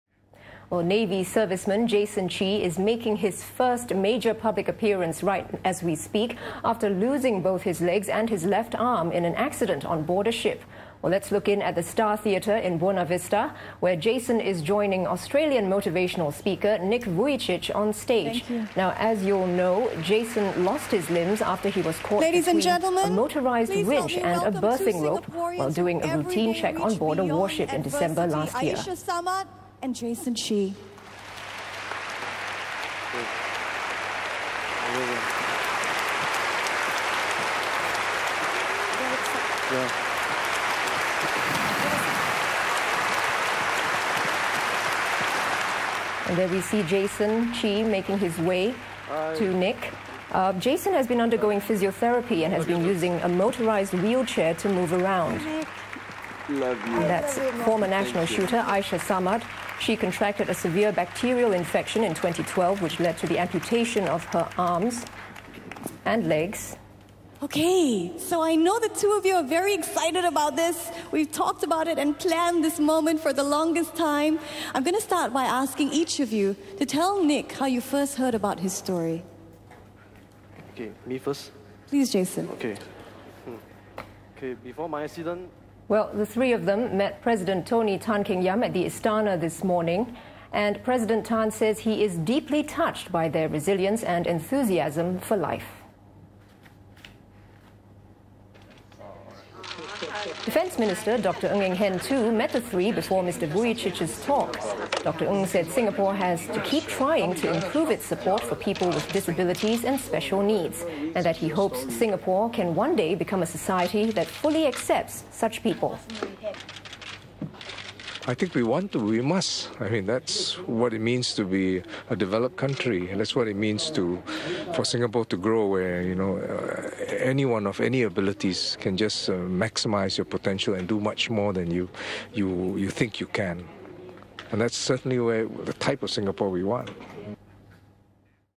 Jason Chee meets Nick Vujicic - 07Sep2013
SINGAPORE: President Tony Tan Keng Yam met Jason Chee, Aishah Samad and Nick Vujicic at the Istana on Saturday morning.

In a Facebook post, President Tan said in spite their physical limitations, they chose to focus on what they can do instead of what they cannot do.

He said he was deeply touched by their resilience, enthusiasm for life, and passion in pursuing their dreams and in helping others.

Jason Chee, a navy serviceman, lost his legs in a ship accident in December 2012. He also lost his left arm and three fingers on his right hand in the same accident.

Former national shooter Aishah Samad contracted a severe bacterial infection in July 2012 which eventually led to the amputation of her arms and legs.

Nick Vujicic was born without limbs and travels around the world to share his positive outlook on life.